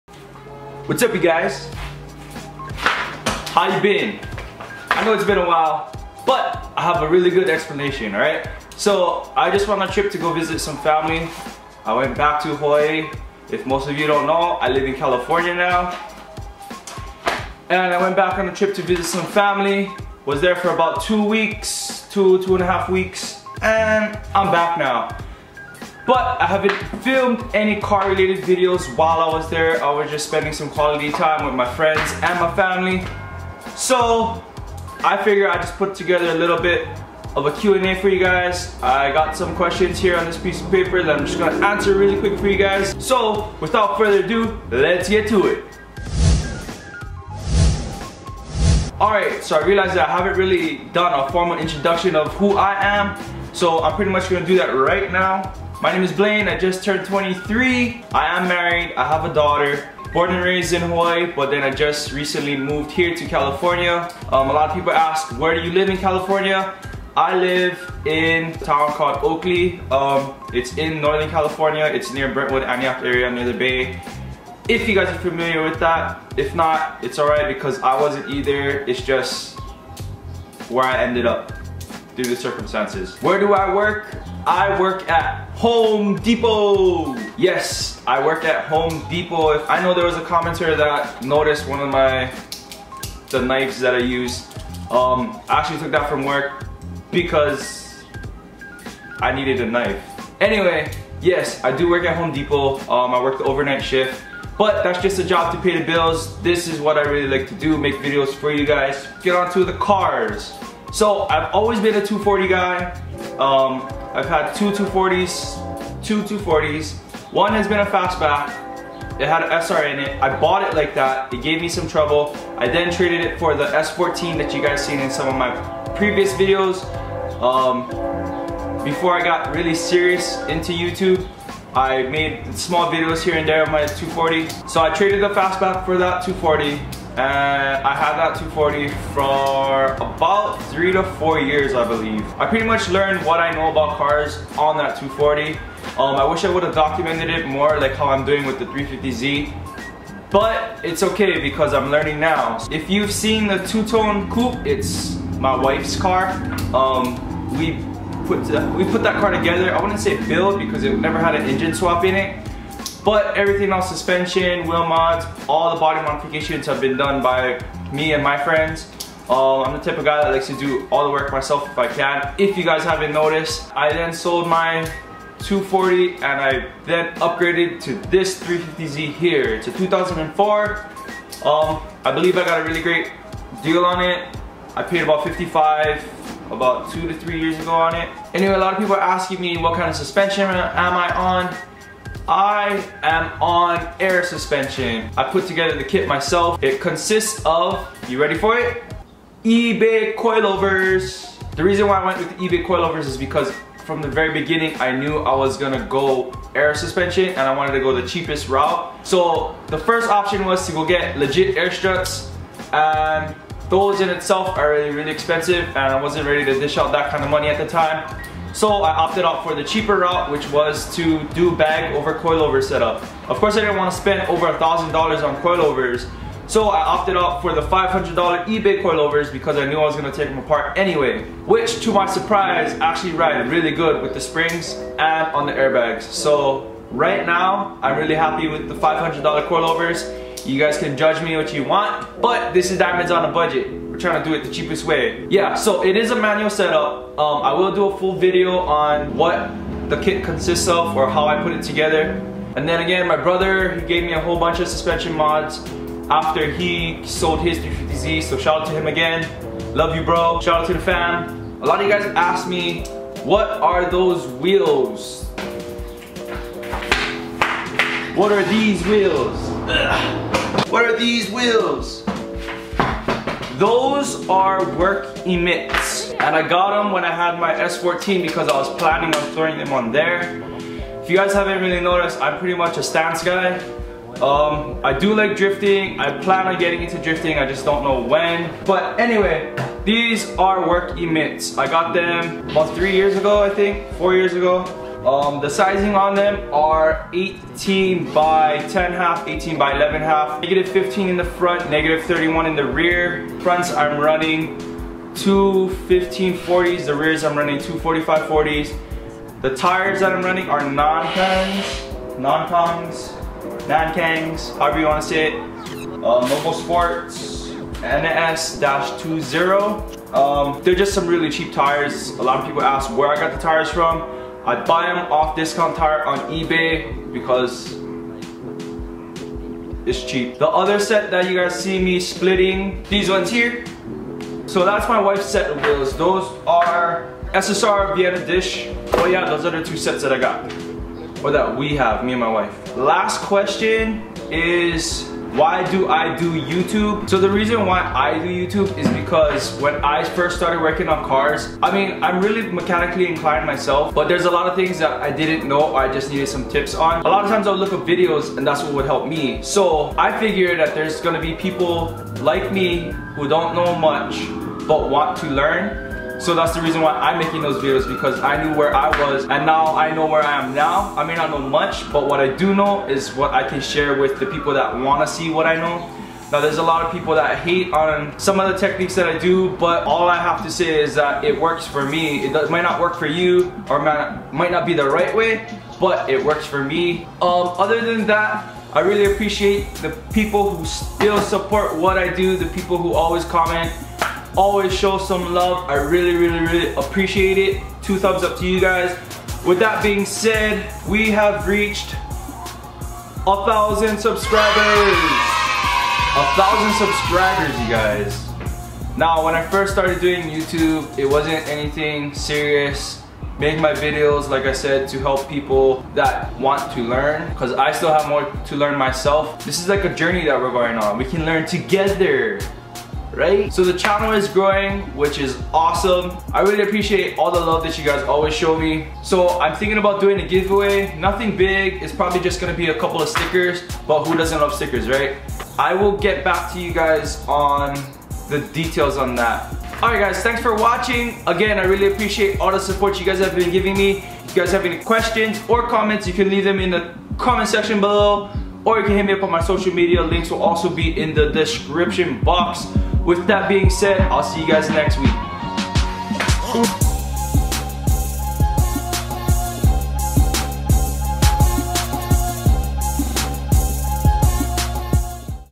Who am I? Q&A
Whats Up Guys, It's Been a while. Sorry for the wait, just got back from Vacation. threw a little Q&A video together for you guys, lmk if you like these kinds of videos. Ill probably do one of these every once in a while, maybe once a month or something.